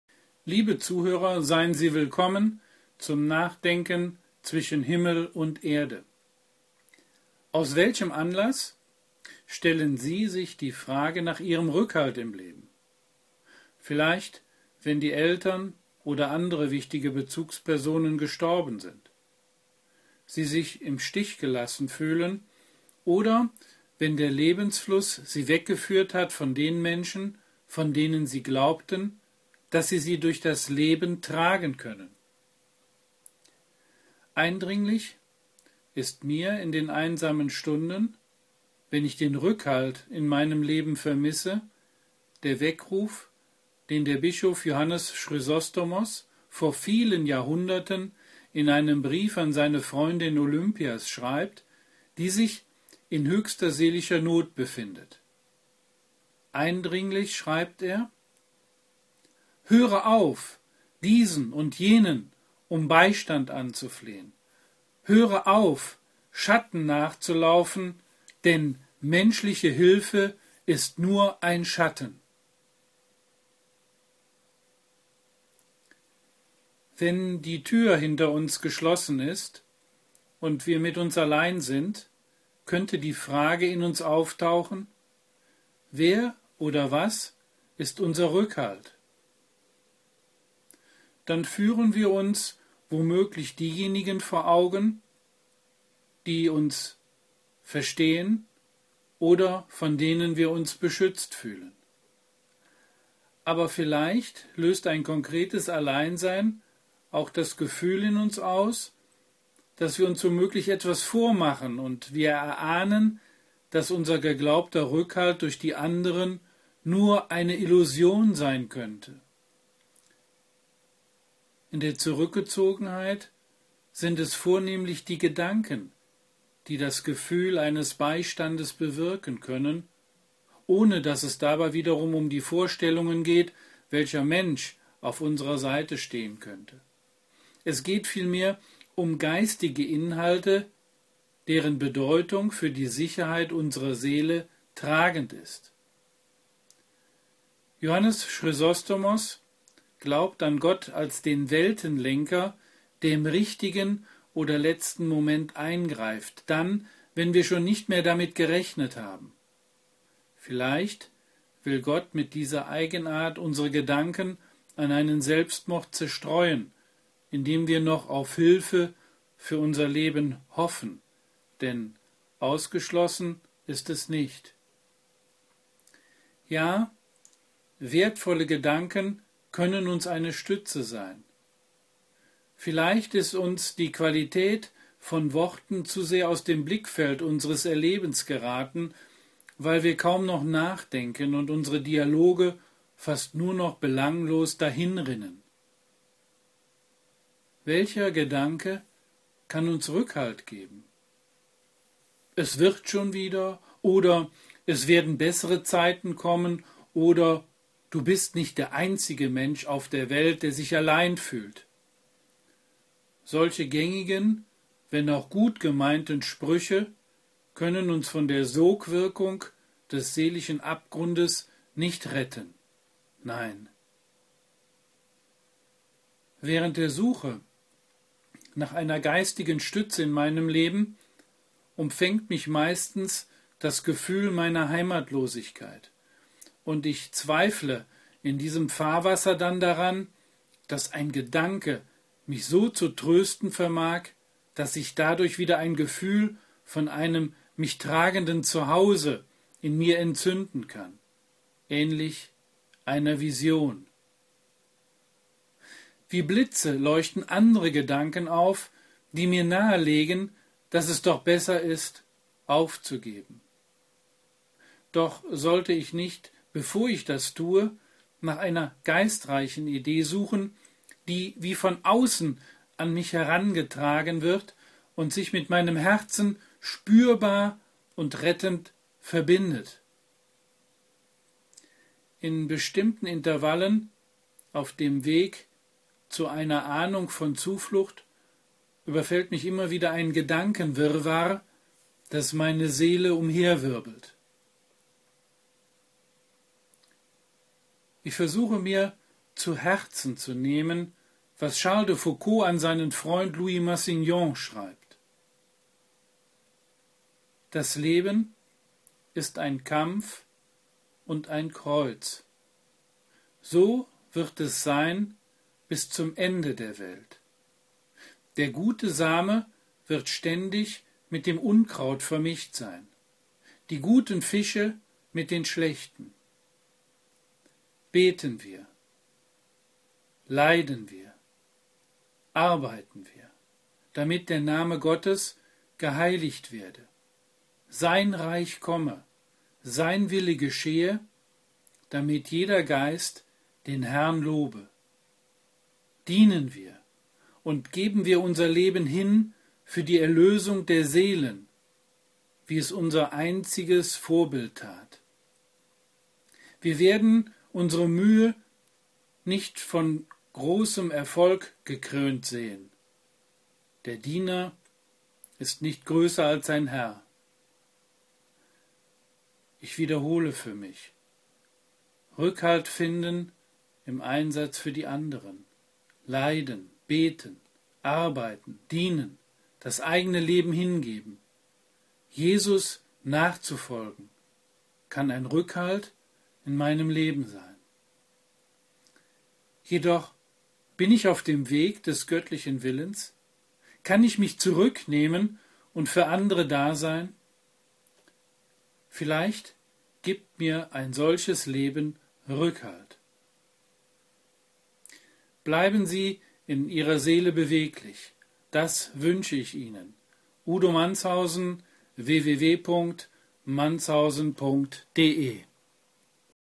Zwischen Himmel und Erde S01E05
Rückhalt

Eindringlich ist mir in den einsamen Stunden, wenn ich den Rückhalt in meinem Leben vermisse, der Weckruf, den der Bischof Johannes Chrysostomus vor vielen Jahrhunderten in einem Brief an seine Freundin Olympias schreibt, die sich in höchster seelischer Not befindet. Er schreibt eindringlich: „Höre auf, Diesen und Jenen um Beistand anzuflehen; höre auf, Schatten nachzulaufen, denn menschliche Hilfe ist nur ein Schatten ..."
In der Zurückgezogenheit sind es vornehmlich die Gedanken, die das Gefühl eines Beistandes bewirken können. Ja, wertvolle Gedanken können uns eine Stütze sein.